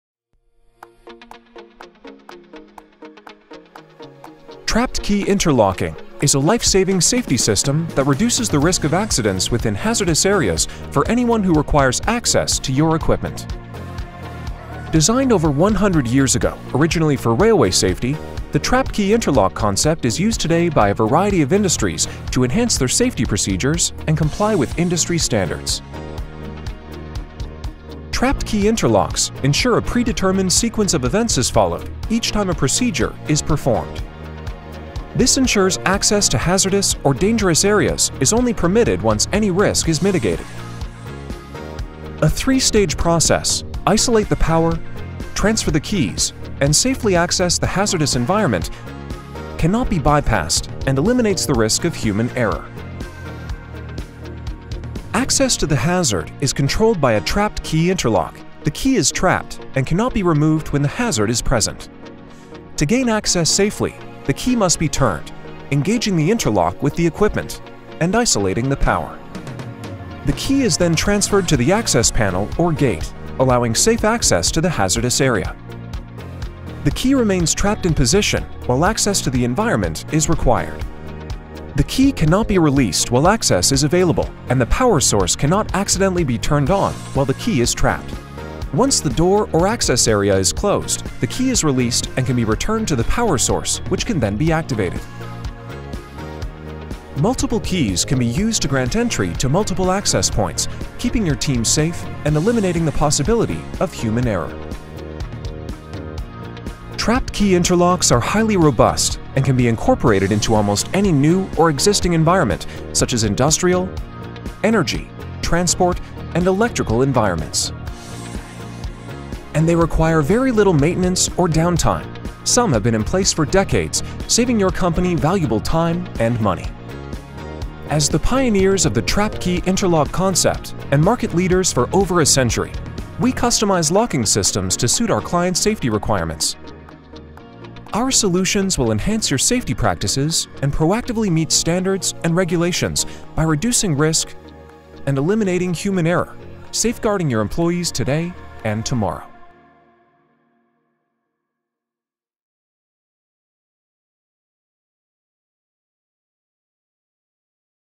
How a Trapped Key Interlock Solution Works
Do you need to know more about reducing risk and eliminating human error in hazardous areas?

Implementing a trapped key interlock safety solution, fix mounted on your equipment and gates/guarding ensures that only after a predetermined sequence of operations is preformed, access into the hazardous area is allowed.  A trapped key safety solution will enhance your current safety procedures, maximizing your protection as well as other employees' when performing procedures around dangerous machinery or equipment.

When it comes to safety, there's no such thing as too many layers of protection. KIRK® interlock solutions mitigate personnel from circumventing safety processes crucial to the defined safety procedure. KIRK® trapped key interlock solutions fully integrate your safety procedures, are simple to operate, and eliminates human error.

There is no compromise when it comes to safety!  Enhance your safety procedures with a trapped key interlock solution from KIRK®.

Check out our video to learn more about how a trapped key solution works and how a solution could benefit your facility’s safety.